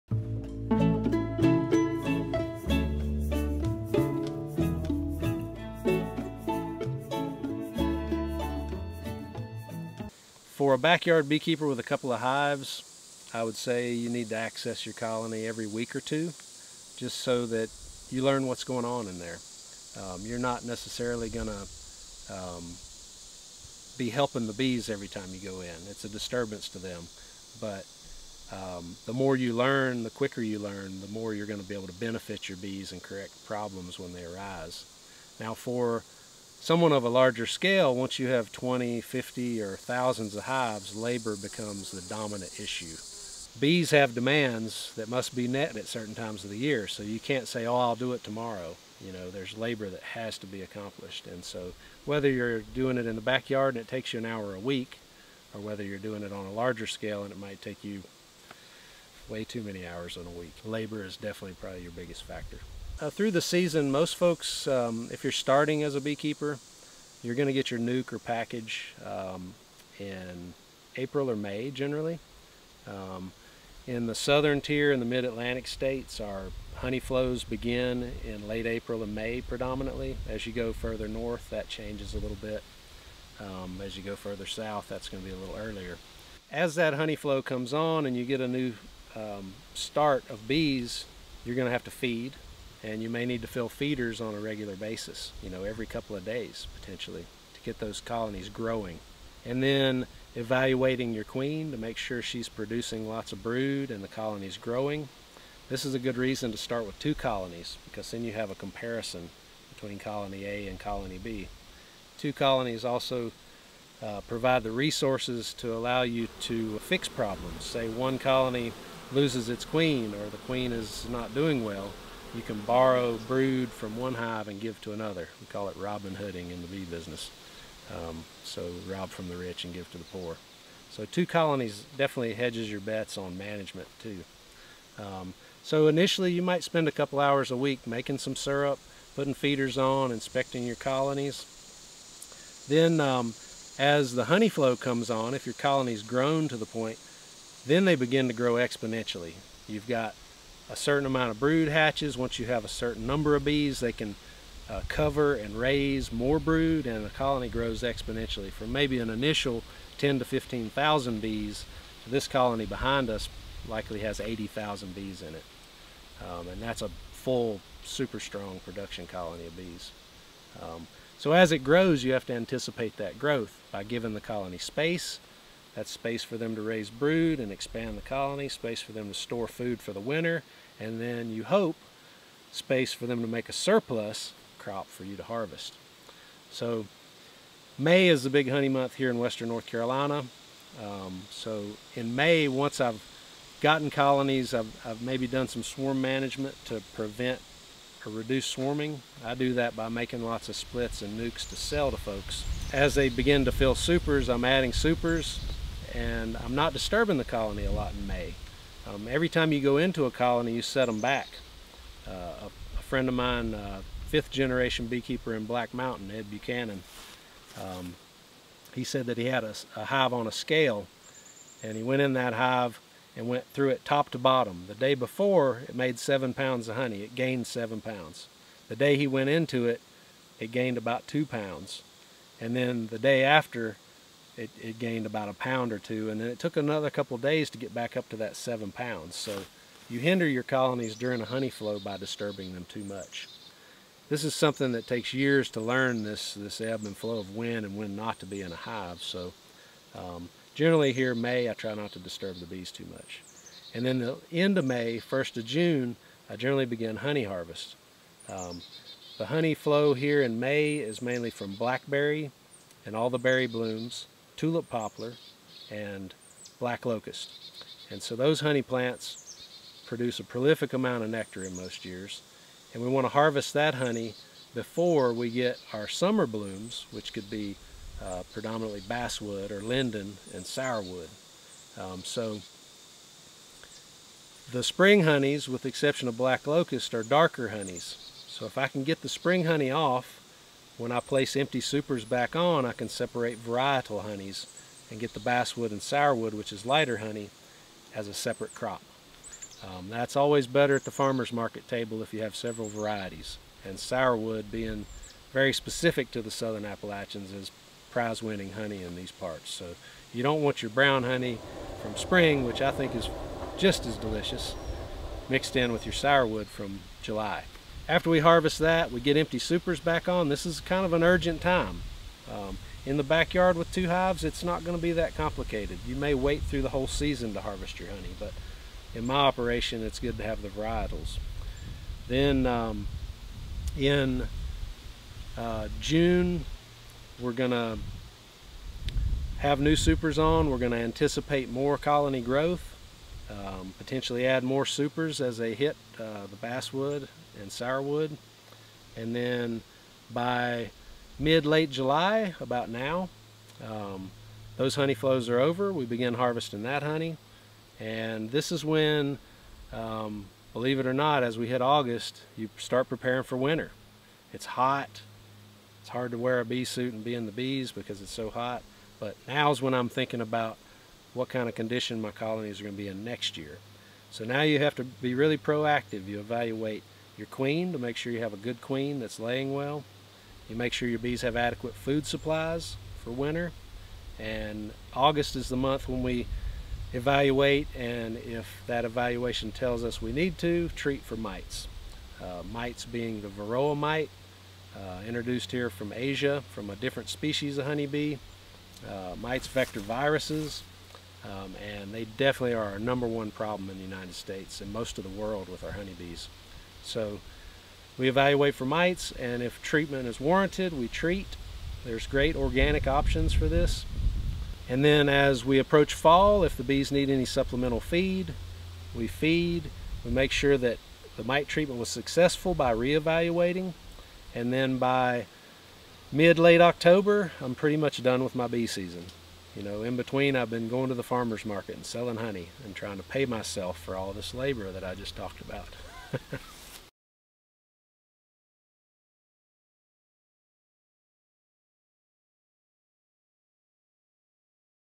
Hive Maintenance and Honey Collection
Jon Christie, owner of Wild Mountain Bees, explains hive maintenance and honey collection through the course of a year. For back yard hobbyists, the job is fairly simple. It's best to start with two hives so that a comparison between hive health and honey production can be made. During flower blooms and honey flows, bees should generally be left alone, as disturbing a bee hive during that time can hinder honey production. Bee hives should be checked periodically however, to ensure that the queen bee is producing brood and the hive is in good working order. A beginning bee keeper generally would obtain a nuc or package in April or May and should feed their hive at that time. Spring blooms should help to boost the hive. At this time, adding more deep honey supers might be a good idea to allow the hive adequate room to grow. If enough honey is produced during this time, it can be collected during the summer. Bee colonies should be left with honey to eat over the winter. While providing feed will help your bee colony, the honey from autumn is best left in the hive to sustain the bees over winter.